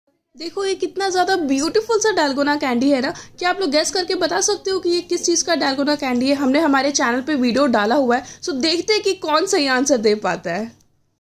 GUESS karo ye kiska DALGONA CANDY hai???
Can you guess ki ye kiska DALGONA CANDY hai?

Did you try this Squid Game Honeycomb Cookie Challenge?

WELCOME TO OUR NEW SHORTS CHANNEL ❤️

xoxo
Anukriti & Swikriti